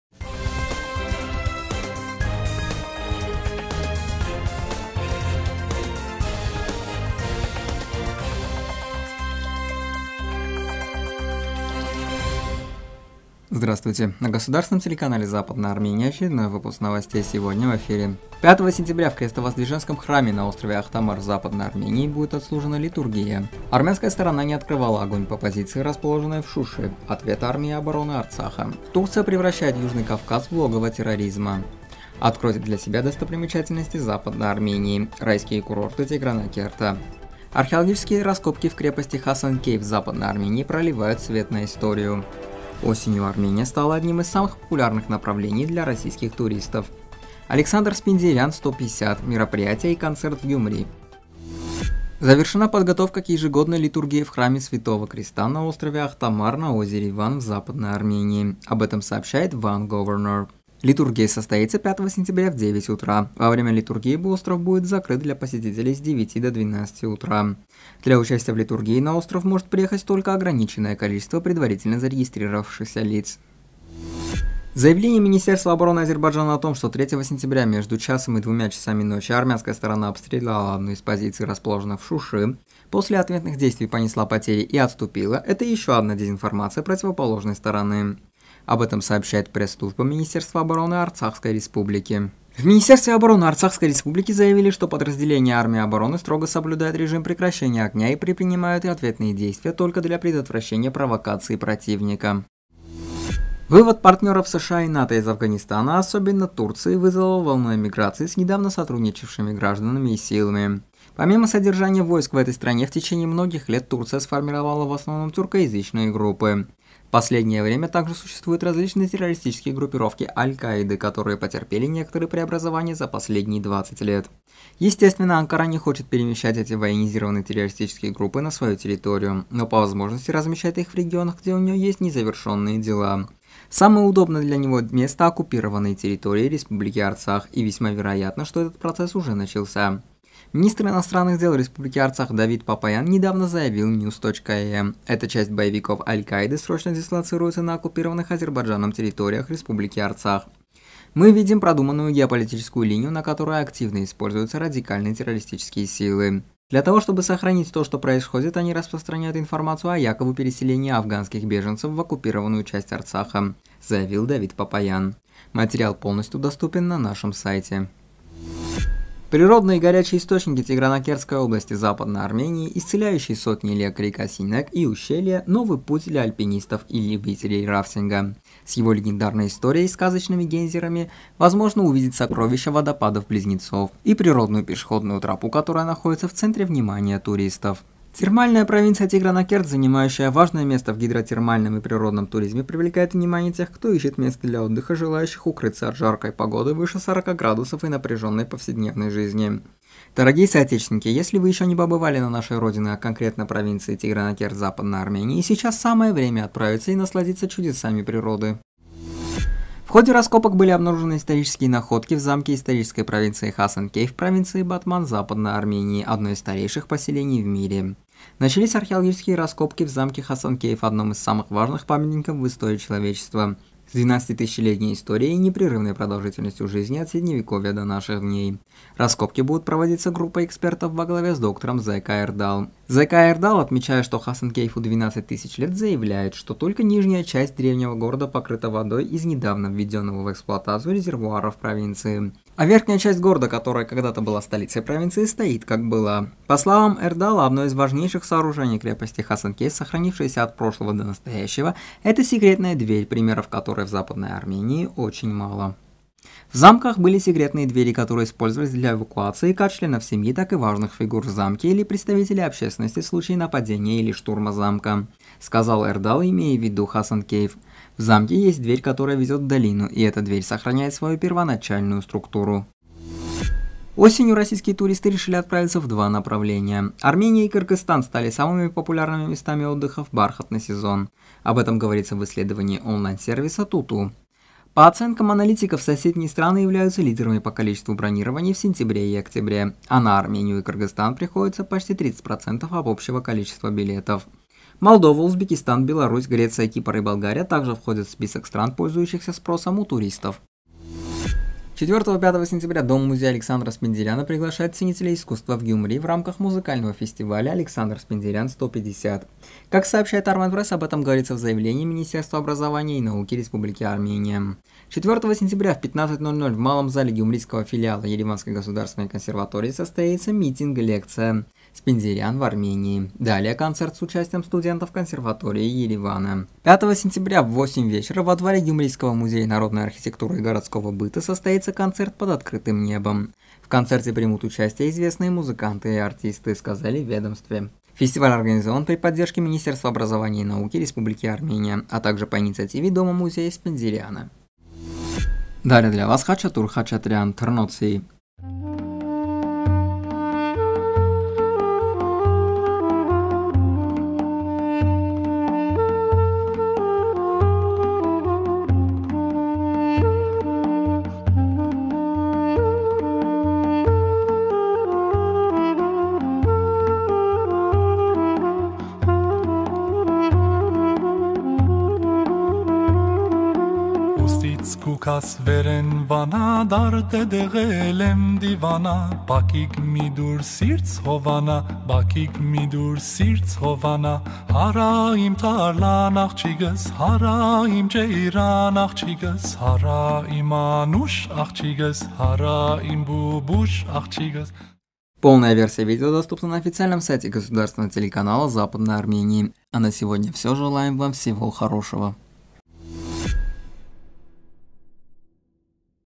Турция превращает Южный Кавказ в логово терроризма.НОВОСТИ 03.09.2021
1. 5 сентября в храме на острове Ахтамар в Западной Армении будет отслужена литургия.
2. Армянская сторона не открывала огонь по позиции, расположенной в Шуши. Ответ Армии обороны Арцаха
3. Турция превращает Южный Кавказ в логово терроризма.
4. Откройте для себя достопримечательности Западной Армении - райские курорты Тигранакерта.
5. Археологические раскопки в крепости Хасанкейф Западной Армении проливают свет на историю.
6. Осенью Армения стала одним из самых популярных направлений для российских туристов.
7. «Александр Спендиарян-150». мероприятия и концерт в Гюмри